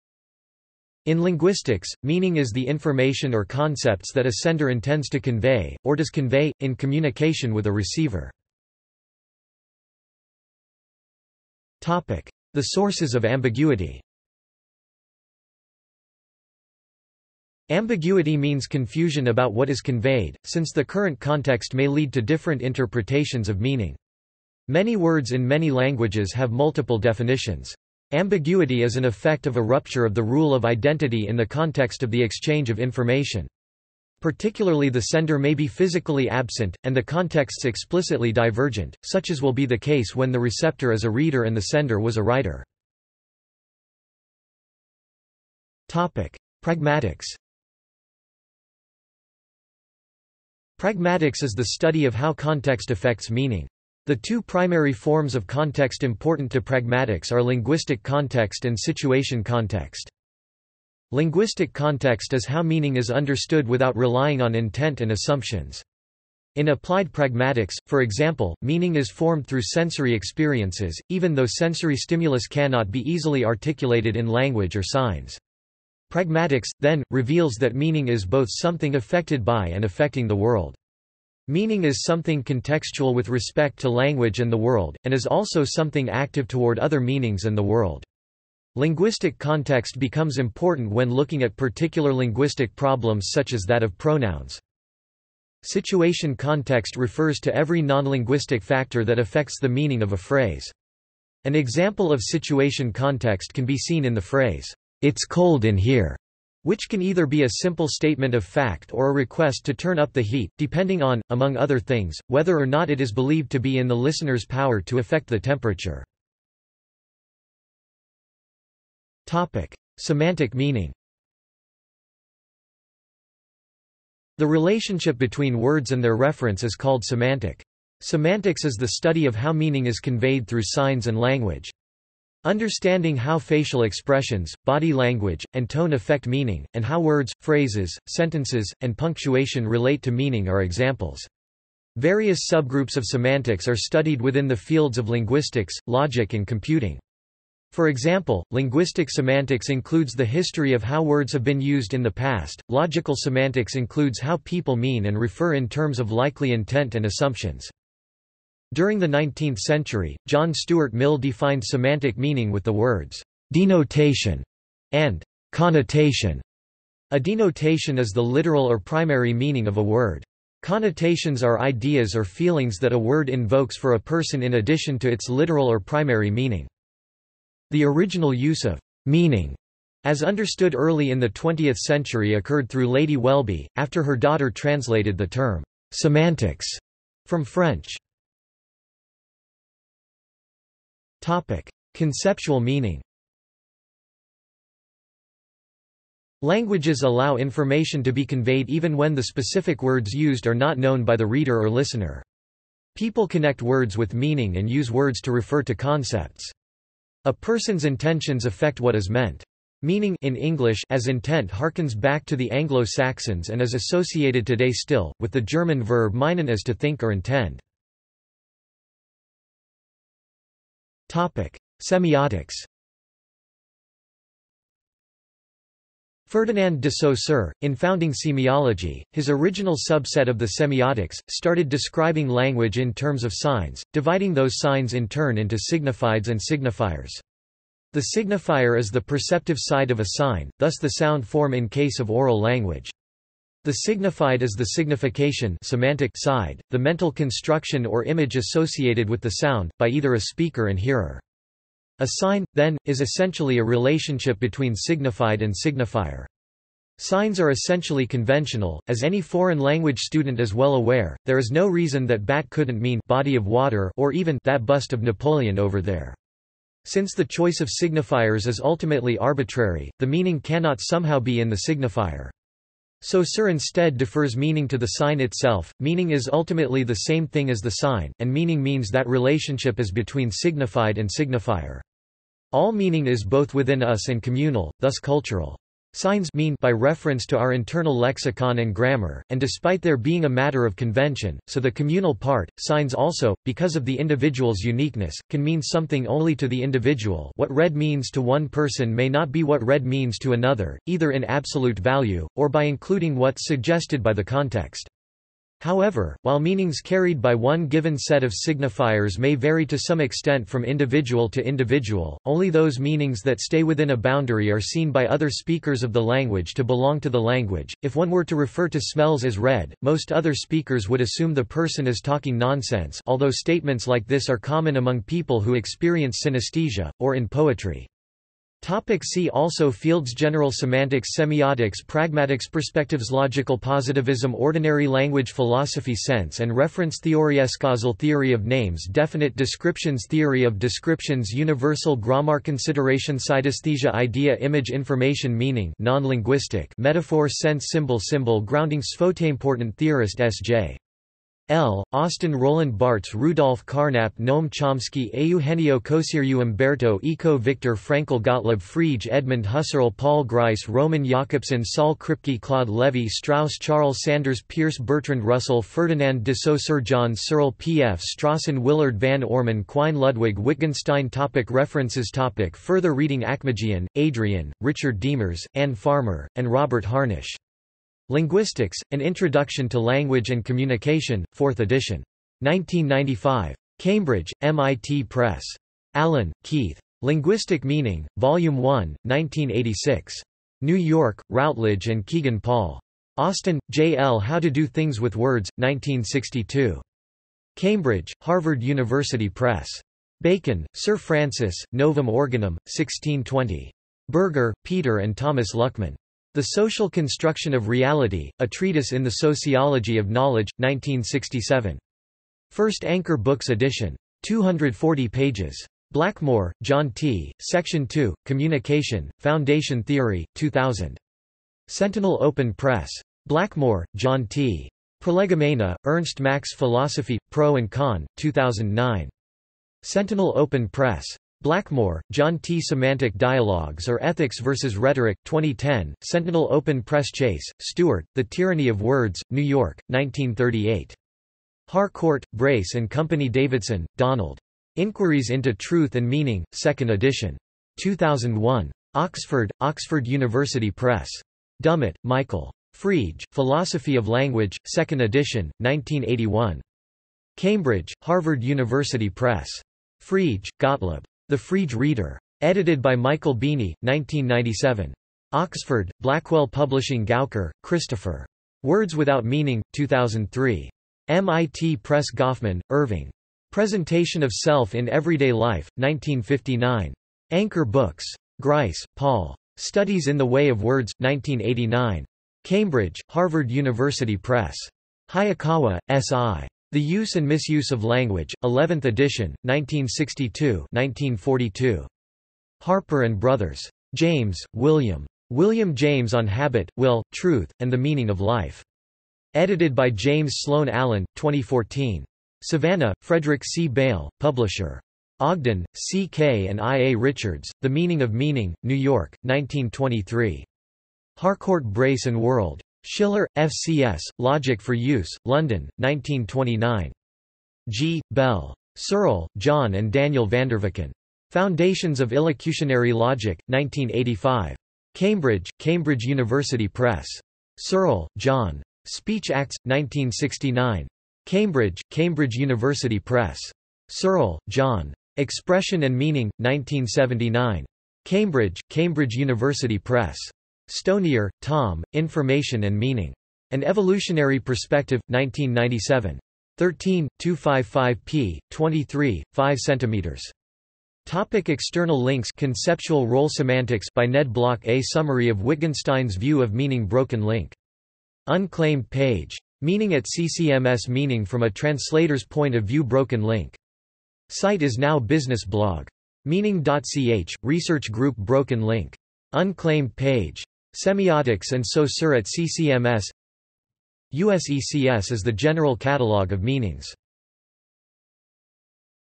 Meaning (linguistics) | Wikipedia audio article
This is an audio version of the Wikipedia Article:
Meaning (linguistics)

- Socrates

SUMMARY
In linguistics, meaning is the information or concepts that a sender intends to convey, or does convey, in communication with a receiver.